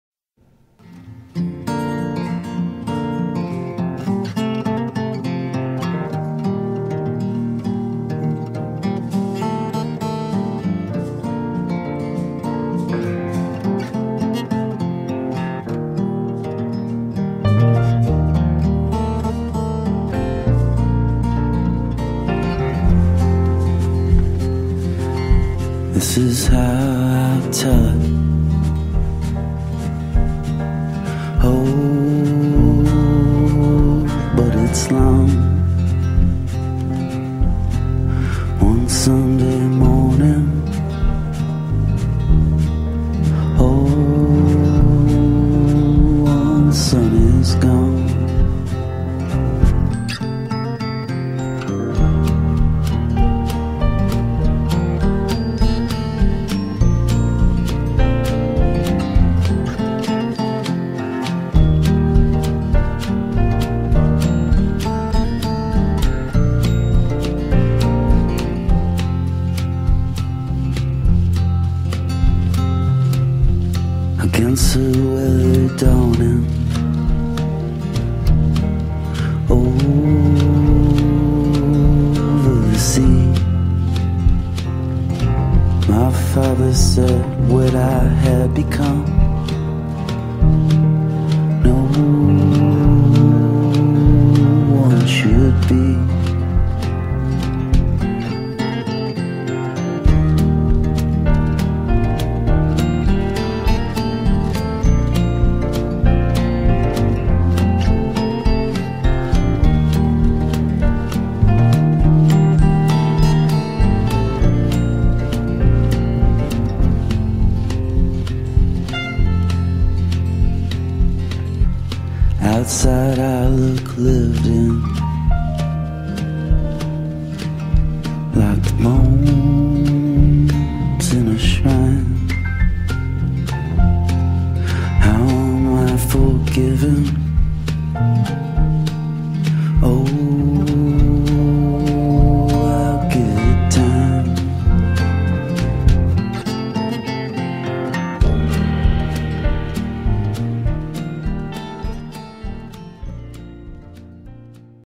One Sunday Morning...
Timelapse of a fall Sunday in Berlin.